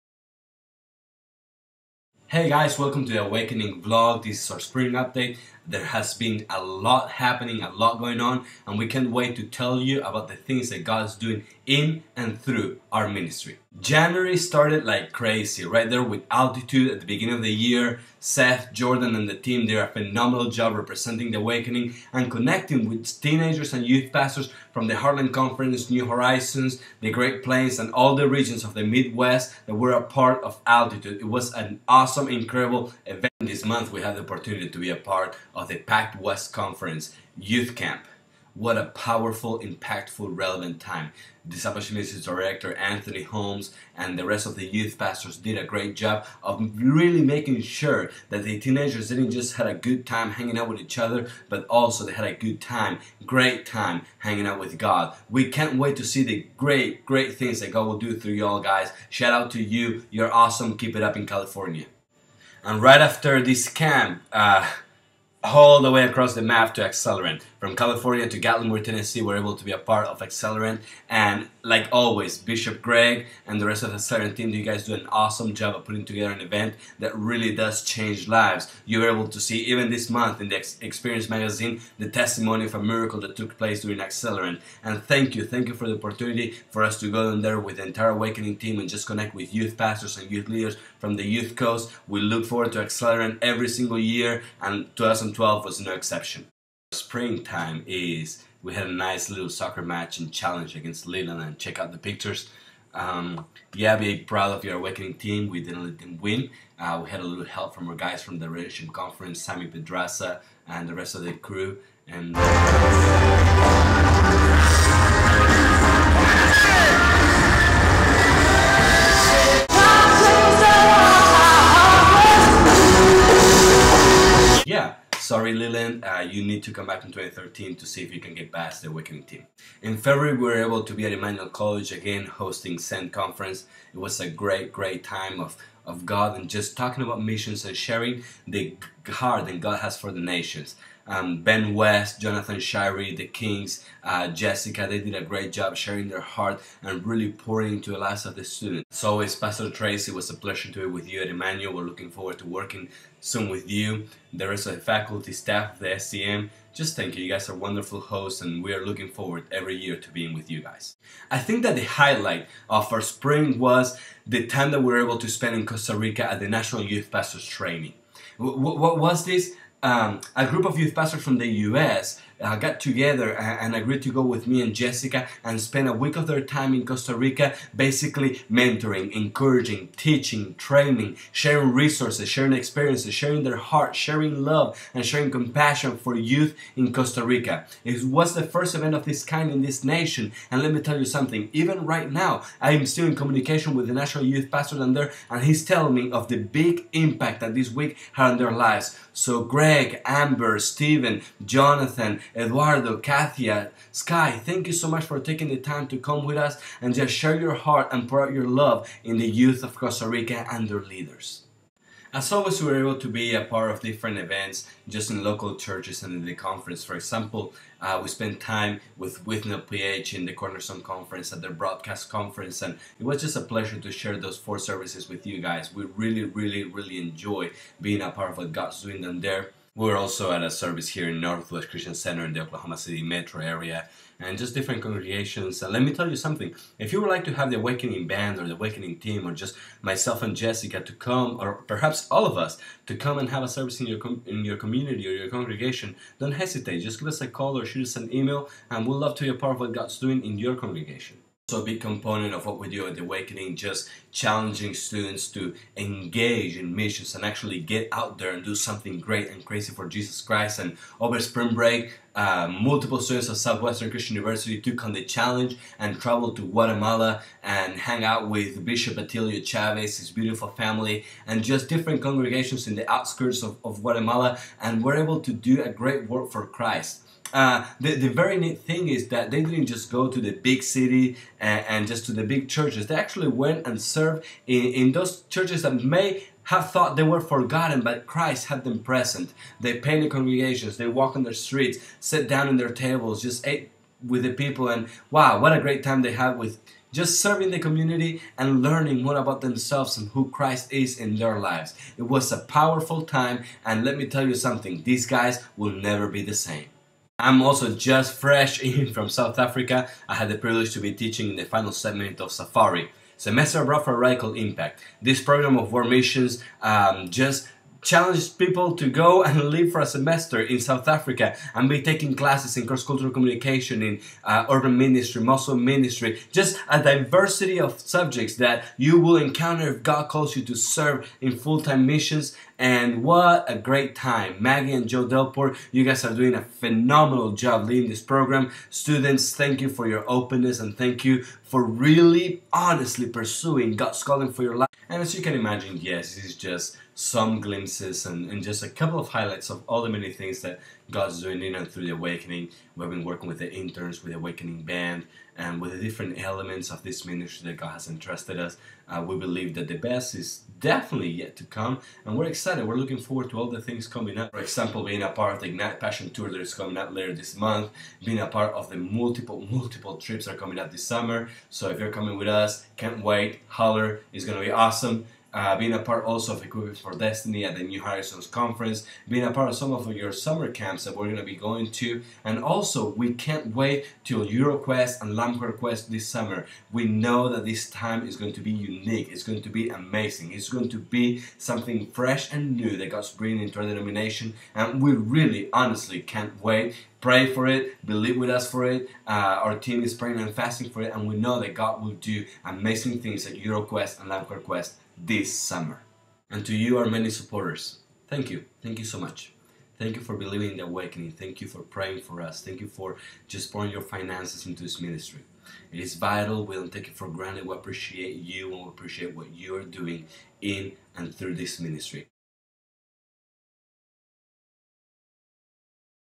Spring 2012 Recap
just a quick update of what's going on at The Awakening right now and what we have been up to through out the Spring!